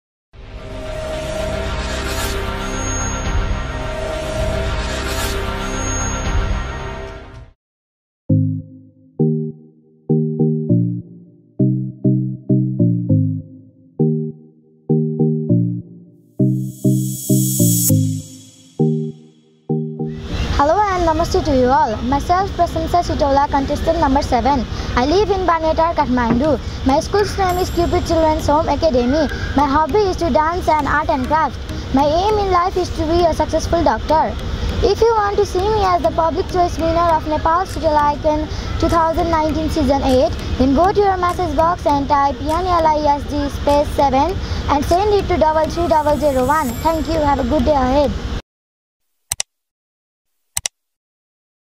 Prashamsa Sitaula | Contestant No 7 | Nepal's Little Icon 2019 | ANP Media House
Vote for supporting him at Nepal's Little Icon 2019 Event (Senior Girl). Click Like on the video
For SMS Voting: Type NLISG SPACE  7 and send to 33001.